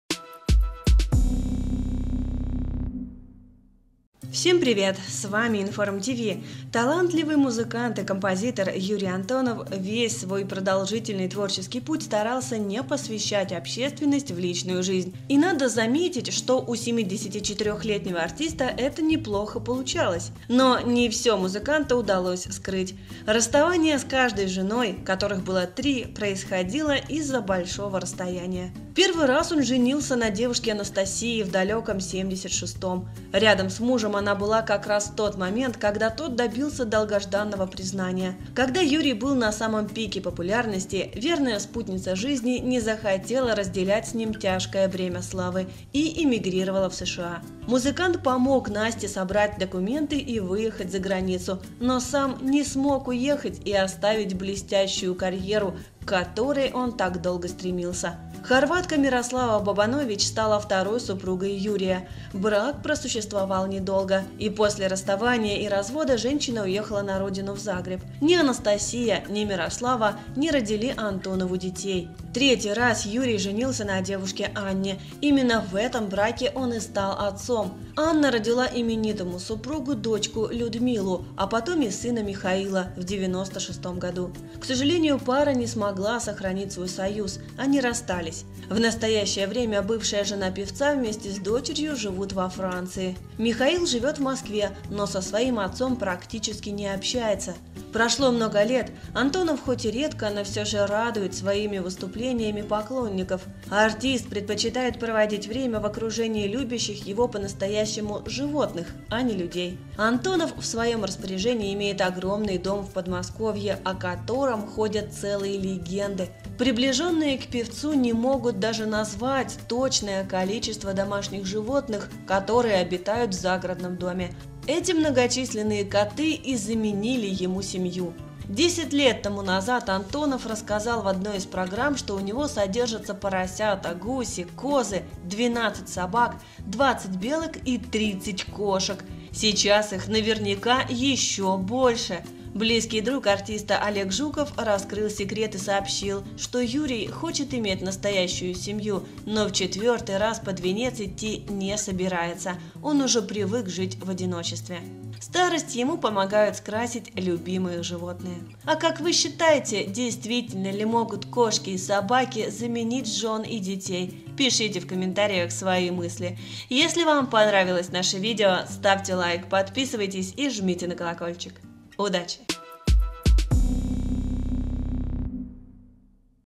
ПОМНИТЕ ПЕВЦА? У НЕГО БЫЛО МНОГО ЖЕН И ДЕТИ, НО ОСТАЛСЯ СОВСЕМ ОДИН
Талантливый музыкант и композитор, любимый многими, Юрий Антонов весь свой продолжительный творческий путь старался не полностью посвящать общественность в личную жизнь. И надо отметить, что у 74-летнего артиста это неплохо получалось.